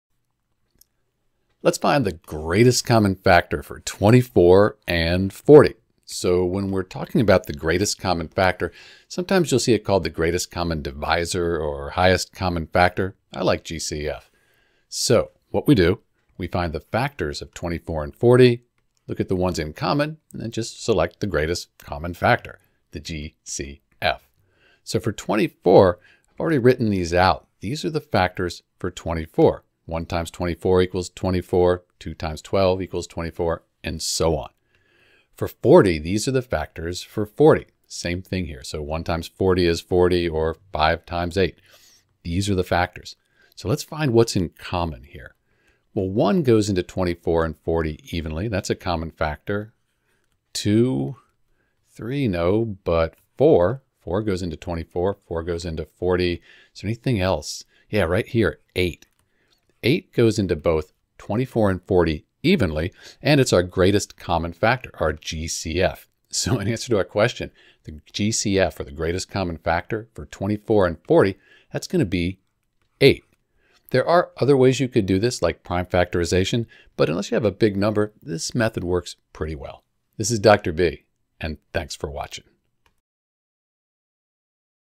How to Find the Greatest Common Factor for 24 and 40
• The factors of 24 are the numbers that can evenly divide 24. In this case, they are 1, 2, 3, 4, 6, 8, 12, and 24.

• The factors of 40 are the numbers that can evenly divide 40. In this case, they are 1, 2, 4, 5, 8, 10, 20, and 40.

Common Factors: 1, 2, 4, 8